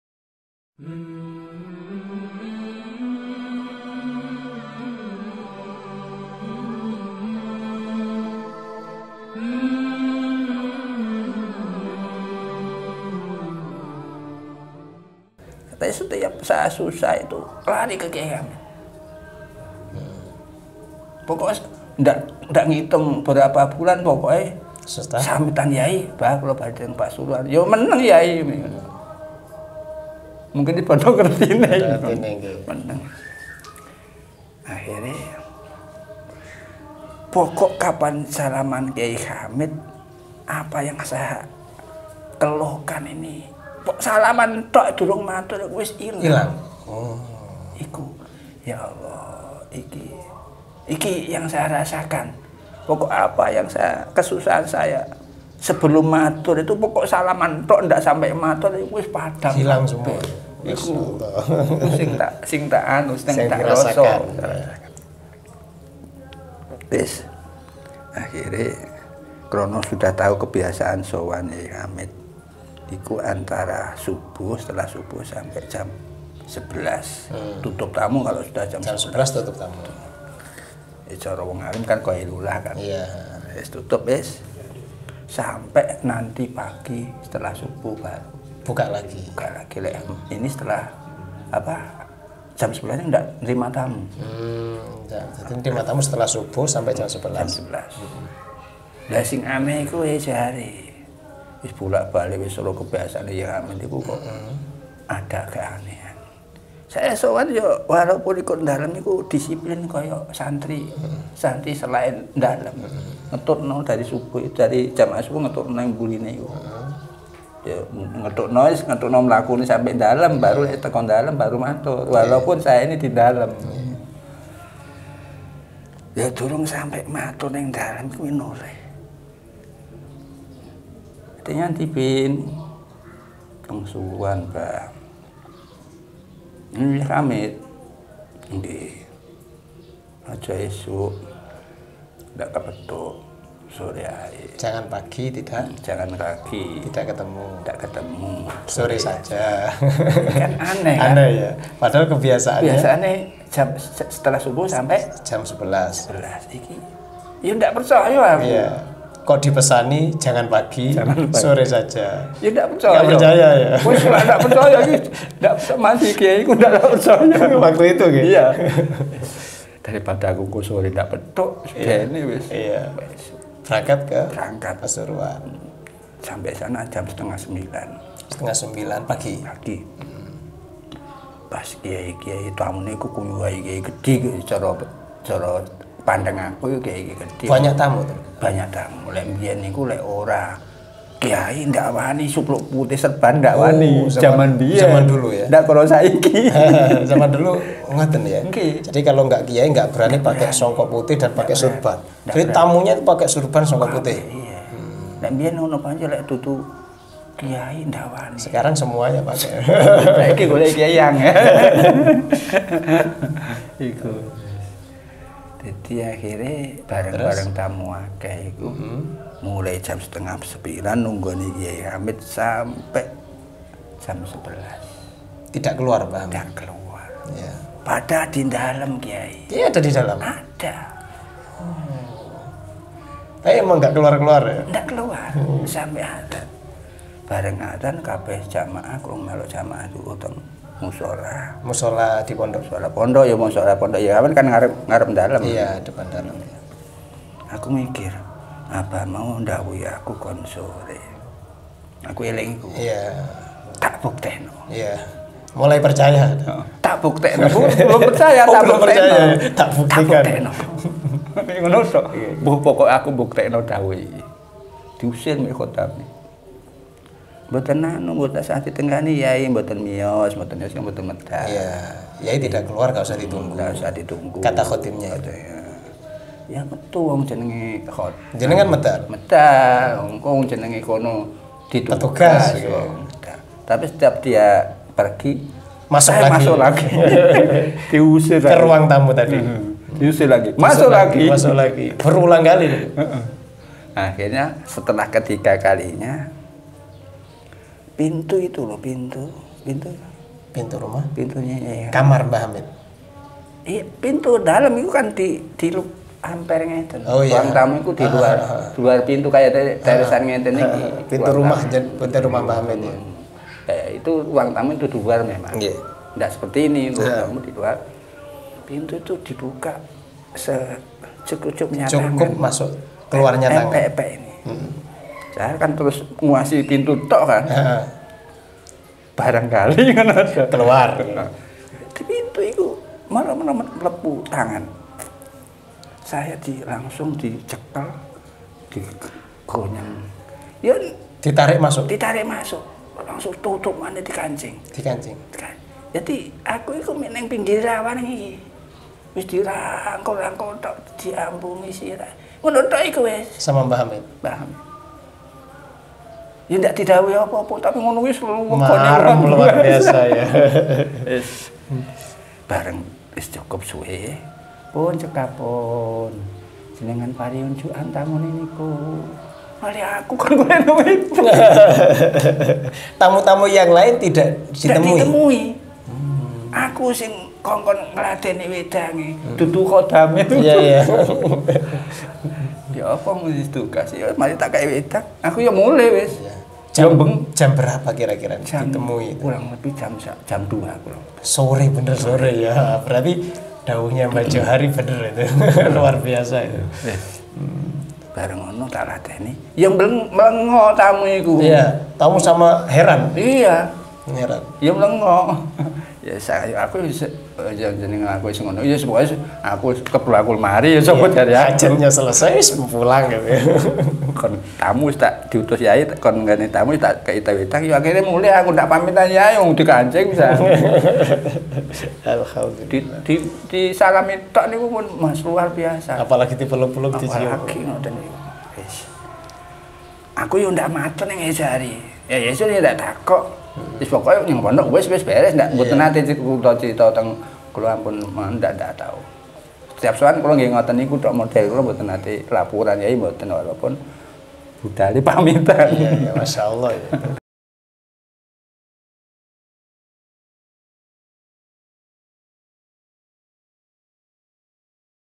KAROMAH KYAI JAUHARI LANGSUNG DARI SAKSI MATA
Bersama Kyai Fathul Mubin (Santri Ndalem Kyai Jauhari Zawawi)

Untuk mensupport Kegiatan Pengembangan Dakwah Gus Rum bisa langsung datang ke kantor komplek Nurul Huda.
Info lebih lanjut silakan dm ke akun instagram @gusrumchannel